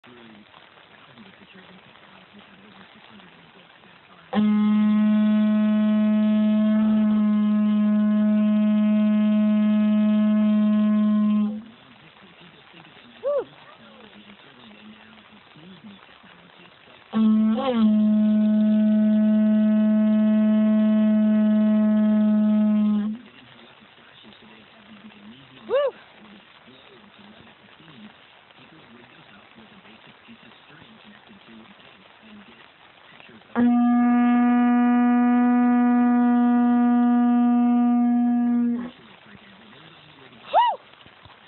Ragnaroc June 18 2016 12:44 pm
I was stuck in the tent aaaaaaall day until the rain stopped. Spasms + pain = crying. So I blew my Conch Shell for Heimdall!

(pronounced “HAME-doll;” Old Norse Heimdallr, whose meaning/etymology is unknown[1]) is one of the Aesir gods and the ever-vigilant guardian of the gods’ stronghold, Asgard.